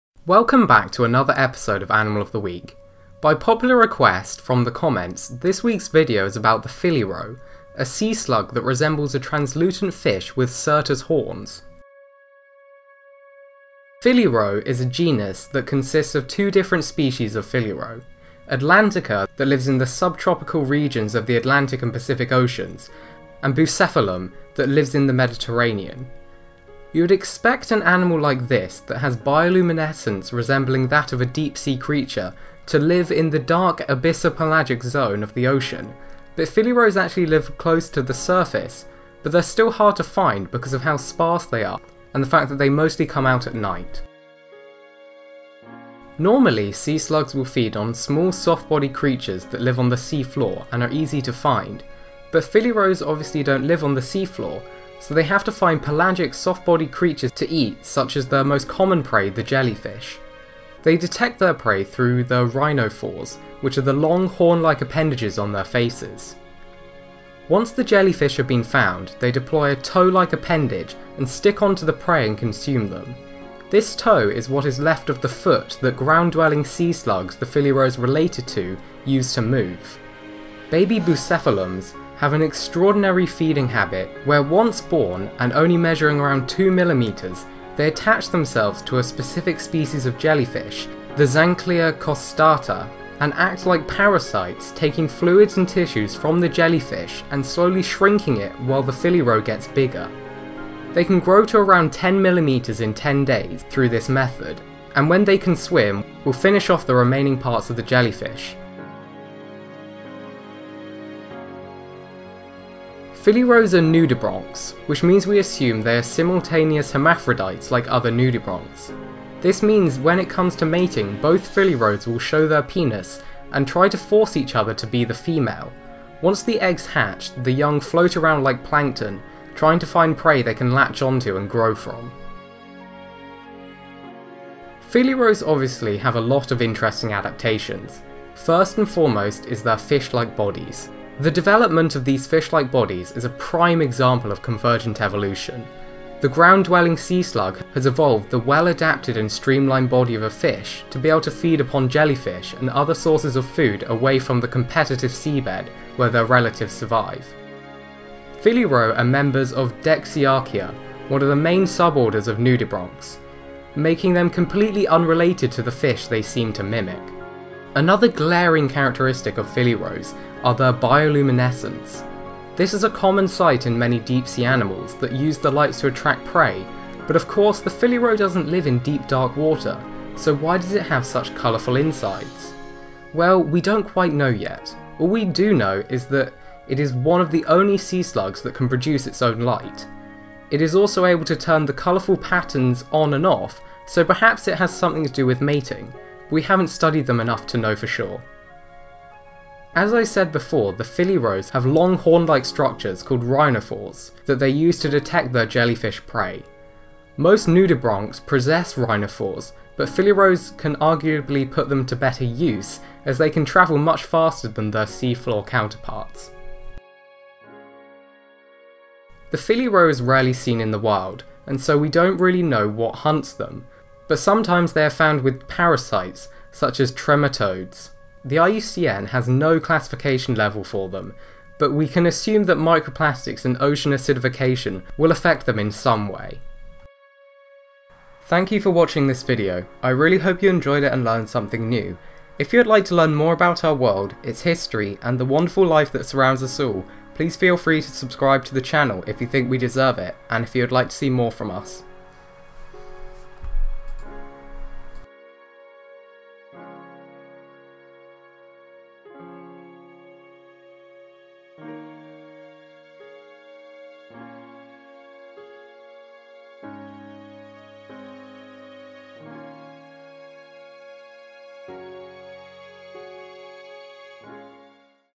Phylliroe - Animal of the Week
This week we're looking at an incredible case of convergent evolution - a nudibranch that looks like a fish. Thanks to everyone who suggested this!